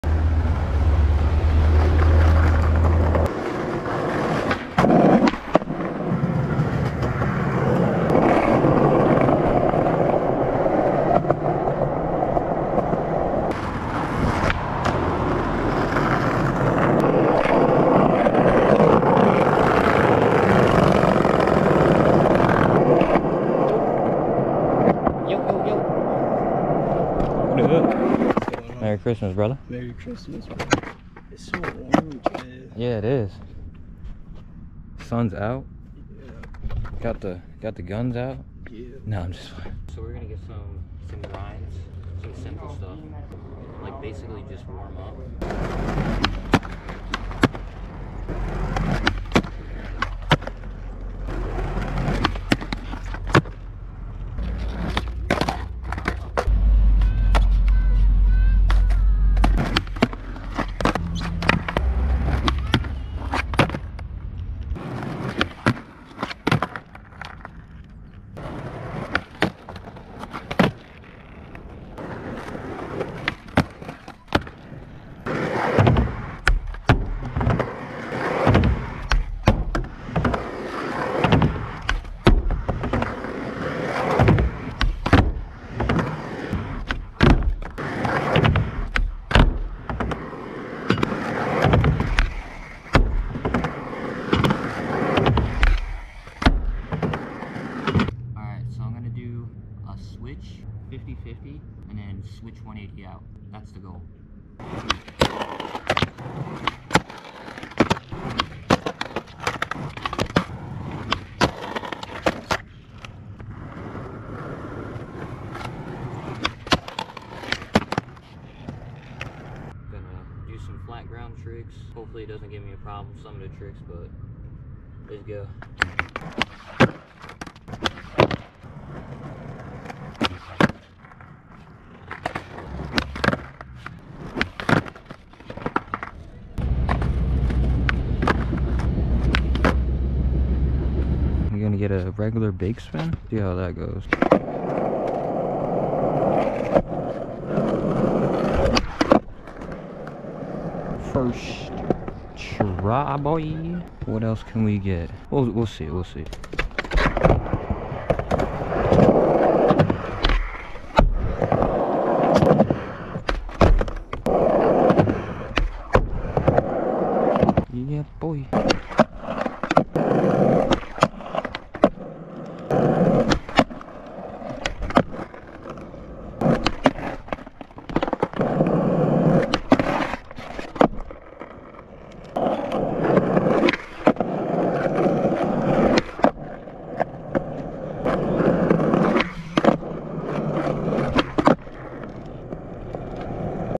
Skated on Christmas Day
This sesh was pretty chill plus it was Christmas ahah I had fun out skating the park and skating with the homie. Hope y’all enjoy the video